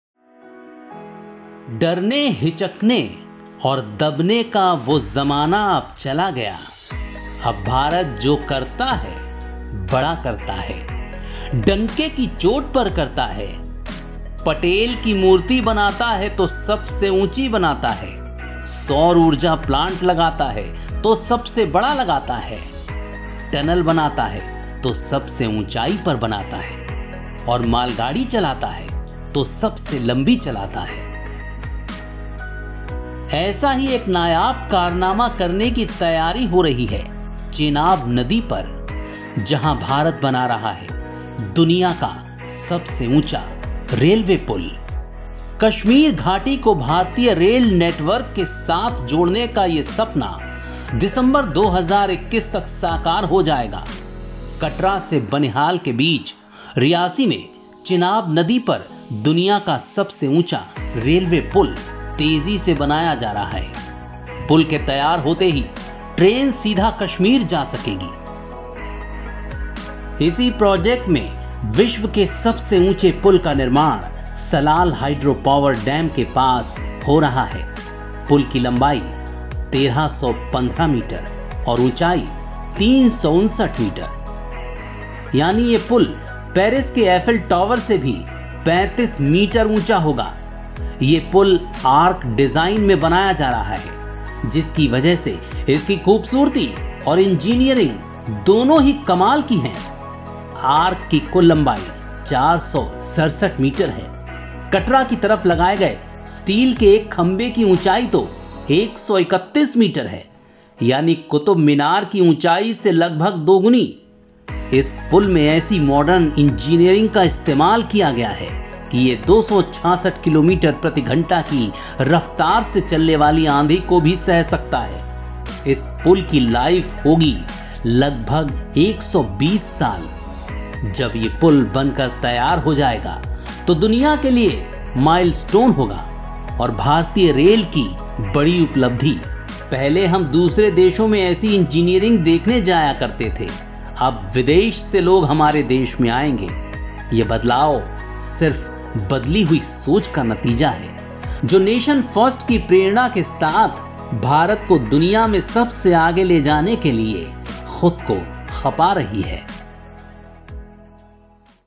Chenab Bridge Latest Update 2021, दुनिया का सबसे ऊचां रेलवे पुल
The Chenab Bridge is an Indian railway steel and concrete arch bridge under construction between Bakkal and Kauri in the Reasi district of Jammu and Kashmir, India. When completed, the bridge will span the Chenab River at a height of 359 m (1,178 ft) above the river, making it the world's highest rail bridge. In November 2017 the base supports were declared completed allowing for the start of the construction of the main arch.
Also watch our another uploaded videos..,